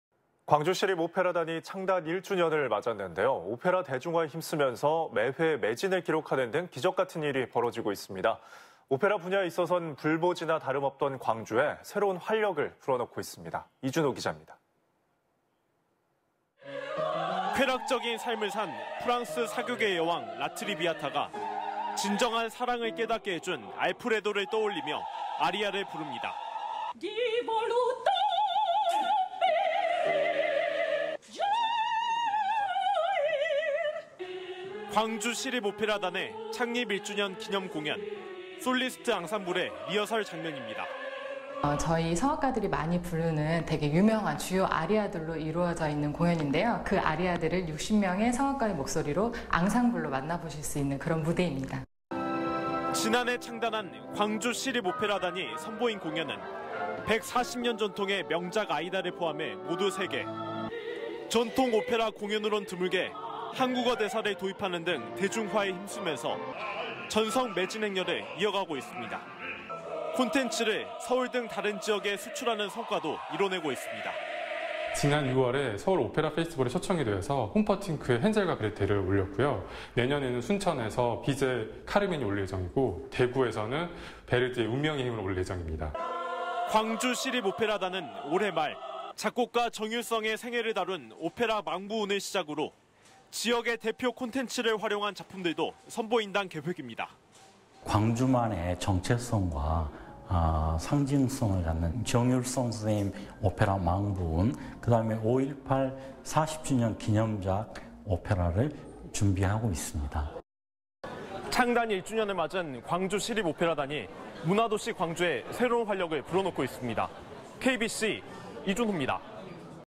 '불모지에서 보여준 가능성'..광주시립오페라단 1주년
광주 시립오페라단이 창단 1주년을 맞았는데요. 오페라 대중화에 힘쓰면서 매회 매진을 기록하는 등 기적같은 일이 벌어지고 있습니다.

오페라 분야에 있어선 불모지나 다름없던 광주에 새로운 활력을 불어넣고 있습니다.

이준호 기잡니다.